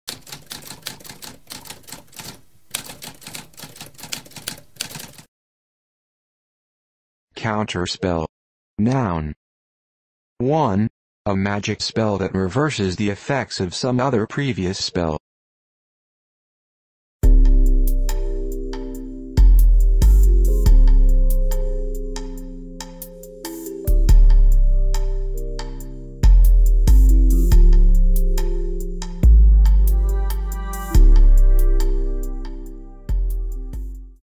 What does counterspell mean?
A spoken definition of counterspell.

Intro Sound:
Typewriter - Tamskp